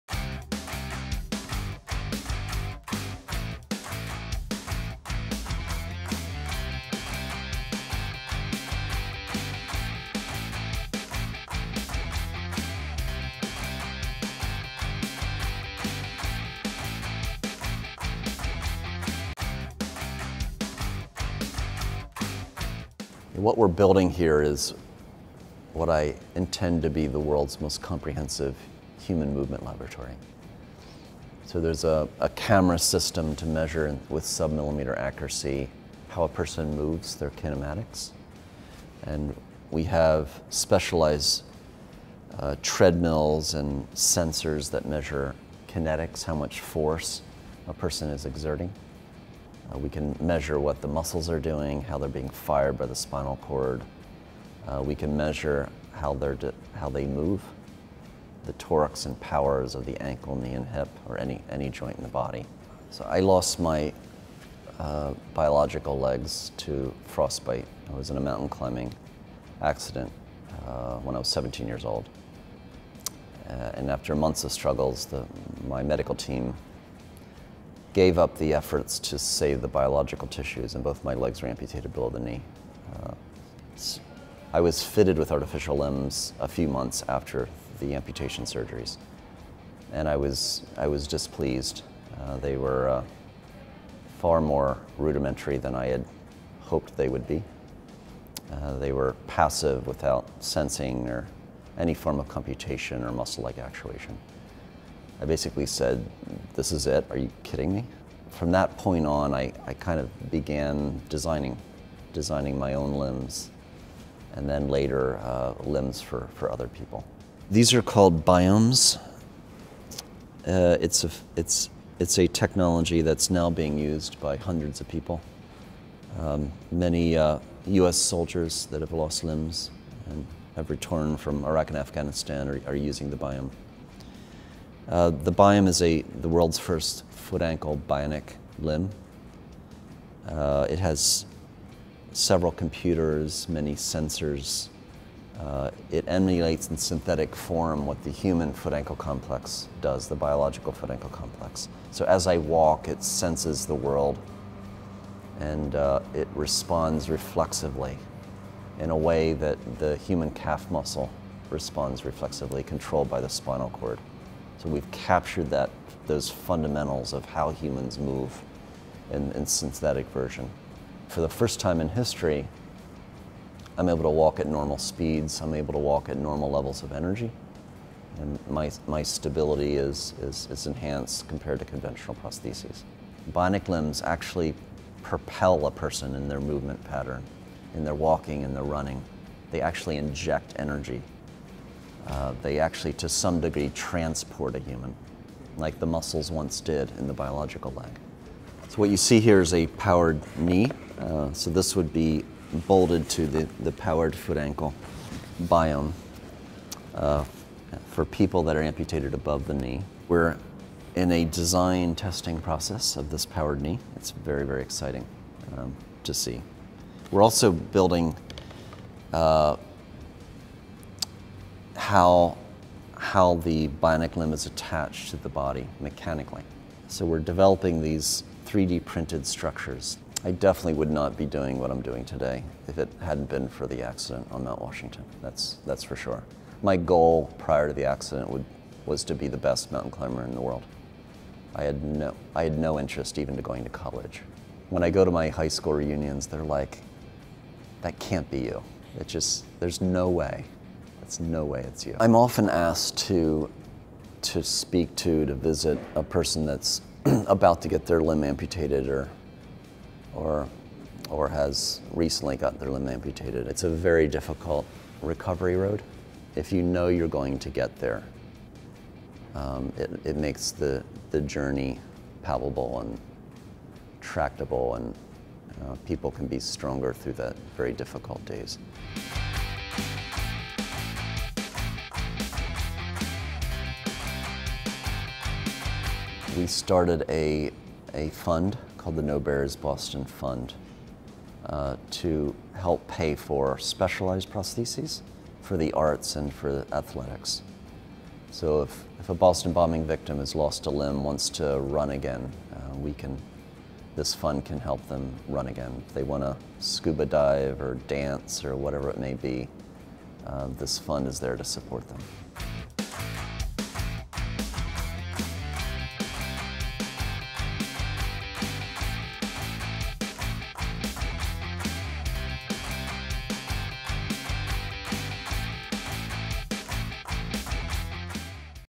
The Innovators: Dr. Hugh Herr, Director, Biomechatronics at MIT
Meet Dr. Hugh Herr, Director of Biomechatronics at MIT. Dr. Herr himself lost both his lower limbs at age 17 in a rock climbing accident.  His passion for climbing drove him to build prosthetics for himself that were more like biological legs.  Now Dr. Herr and his team at MIT design and build the Biom, artificial lower limbs that improve the lifestyle of amputees. The technology that Dr Herr and his team continue to develop goes beyond prosthetics that will continue to change lives.